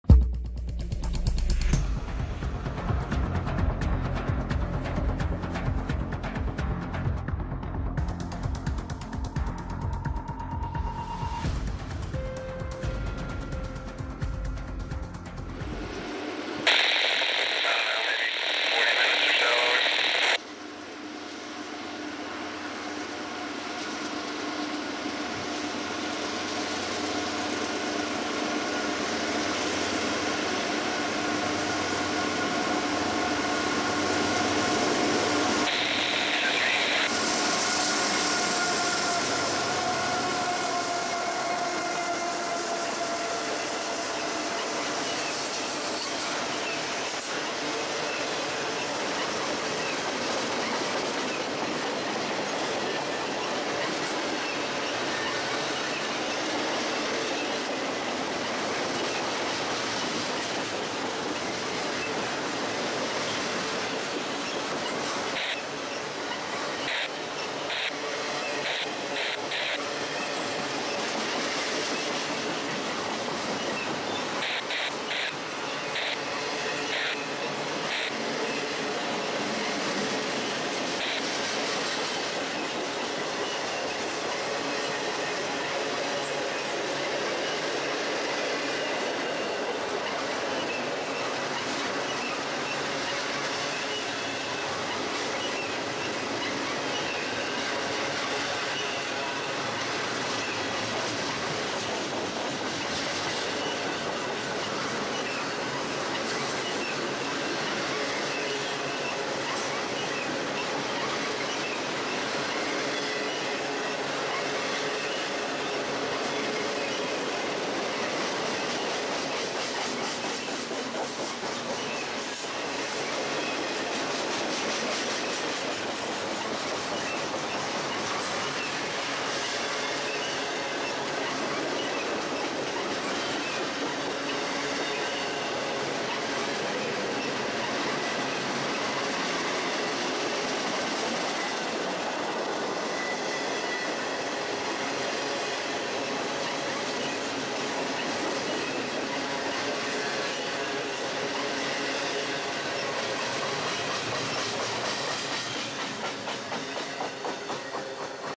NS 26T With AC44C6M Leader
Train: NS 26T (Chicago - 63rd Street, IL to Harrisburg, PA)

Track: Main 1

Length: 71

A rebuilt GE leads a intermodal train charging up the west slope in Cresson.